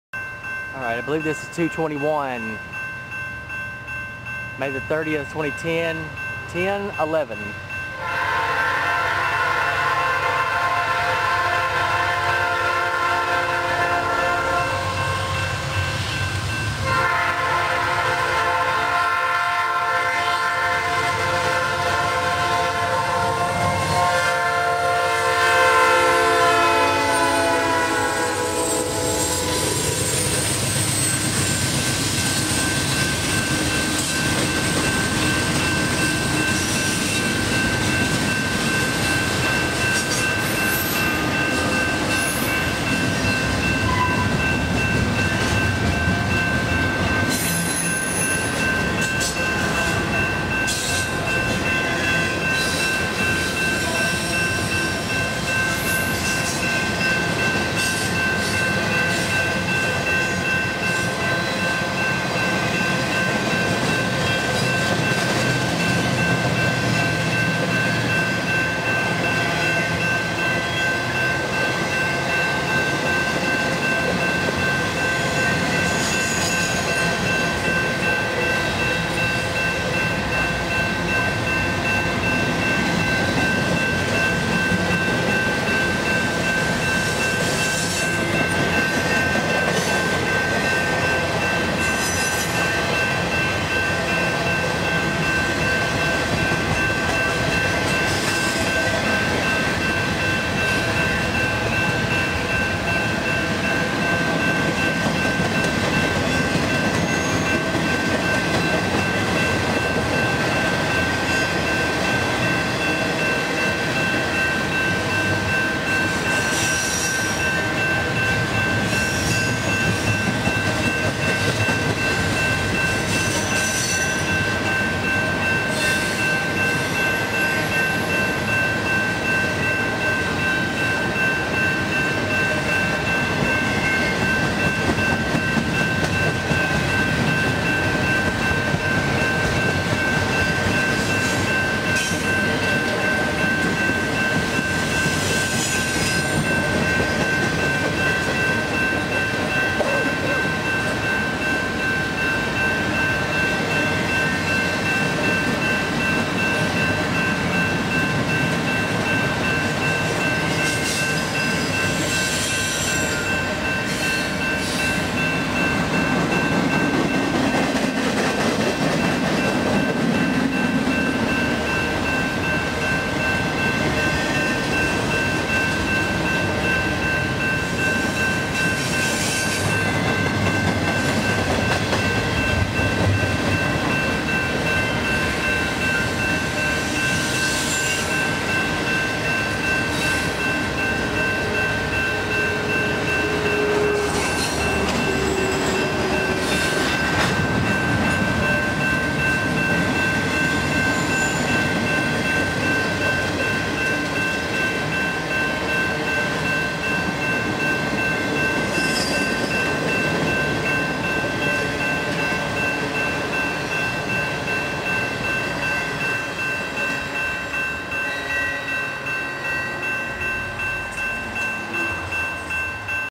Norfolk Southern 221 NB Intermodal in Mableton,Ga 05-30-2010© (16x9)
Norfolk Southern 221 Northbound Intermodal seen here in Mableton,Ga.

Recorded by Gene Hale on May 30, 2010.